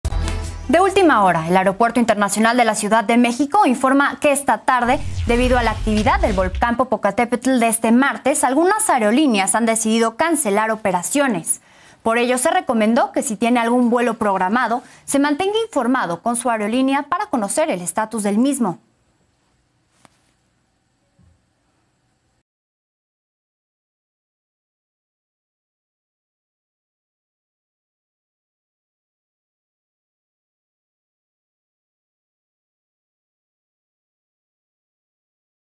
AICM: Aerolíneas cancelan operaciones debido a actividad de volcán Popocatépetl - Las Noticias
El Aeropuerto Internacional de la CDMX informó que debido a la actividad del volcán Popocatépetl de este martes, algunas aerolíneas han decidido cancelar algunas operaciones.